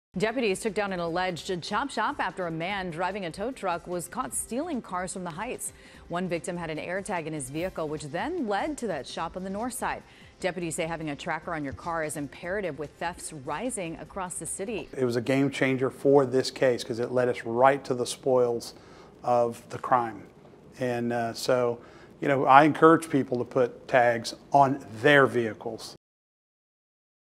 A tow truck, an AirTag, & a chop shop
A suspect was arrested, but authorities worry there could be others involved, as they believe the investigation will reveal other stolen cars linked to the case.